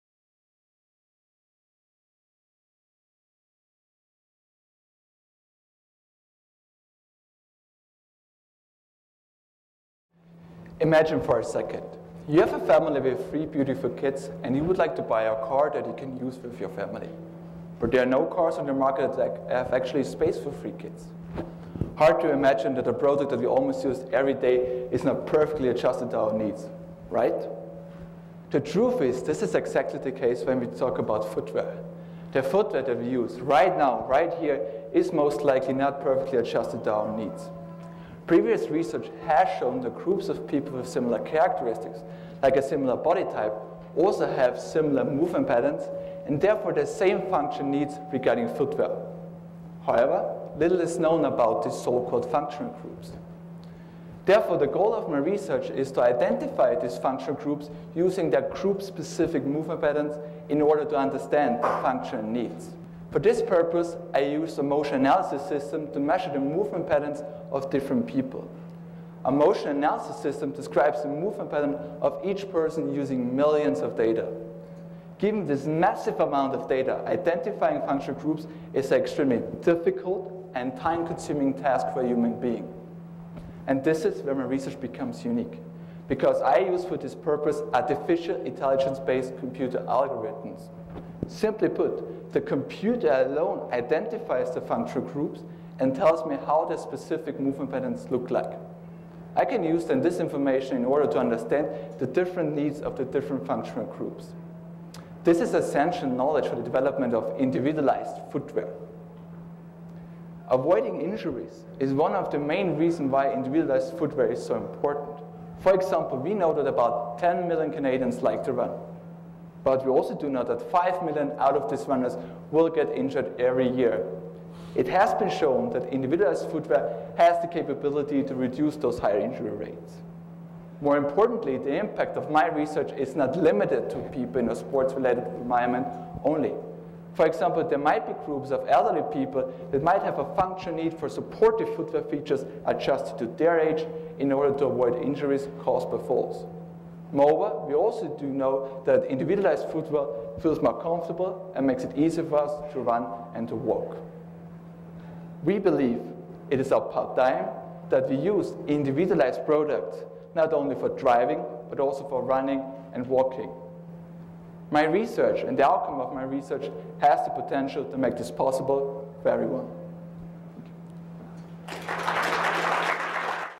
University of Calgary Three Minute Thesis (3MT®) 2014 - Stefan Hoerzer
2014 UCalgary3MT Finalist Stefan Hoerzer from Kinesiology presented "Using artificial intelligence to identify group-specific movement patterns".